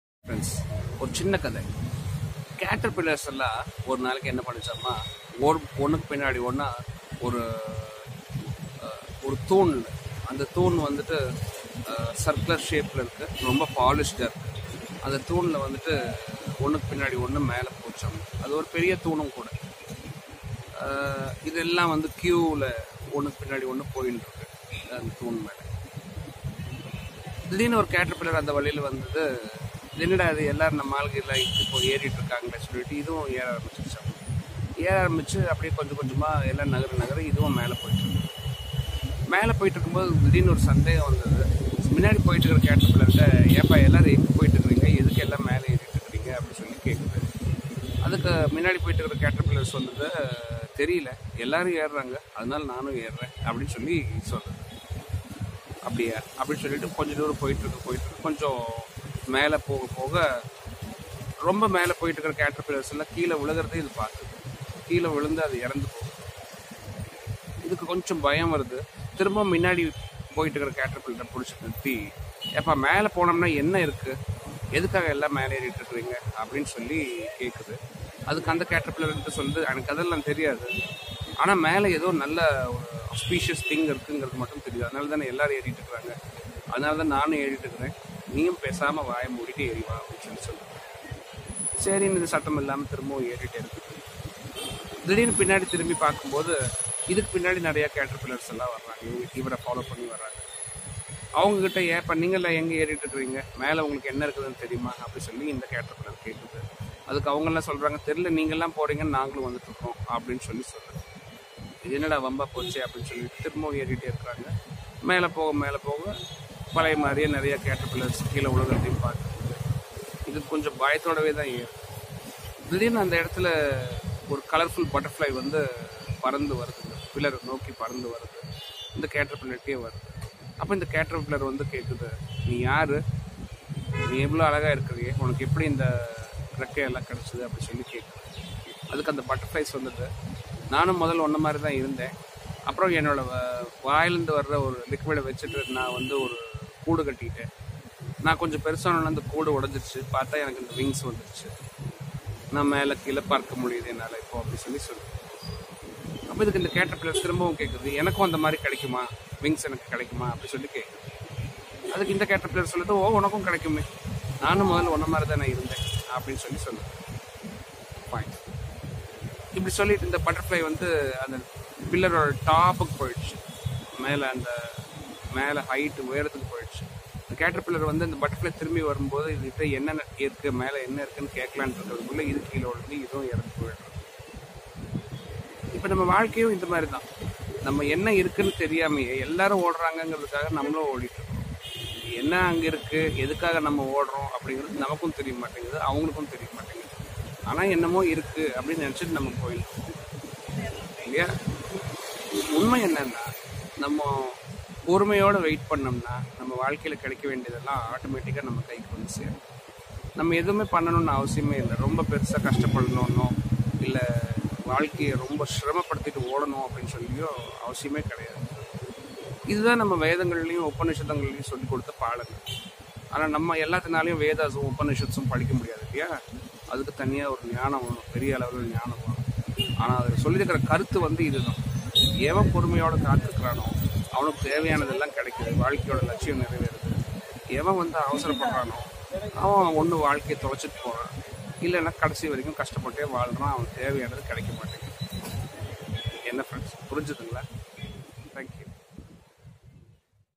வேதங்களும் இதிகாசங்களும் நமக்கு அளித்த வாழ்க்கை பாடங்கள் - Tamil Yugam
வேதங்களும் இதிகாசங்களும் நமக்கு அளித்த வாழ்க்கை பாடங்கள் - Tamil Yugam by Kumaragurubaran

Tamil motivation speech
Motivational speech
Inspiration speech
Motivational speech Tamil
Inspiration speech Tamil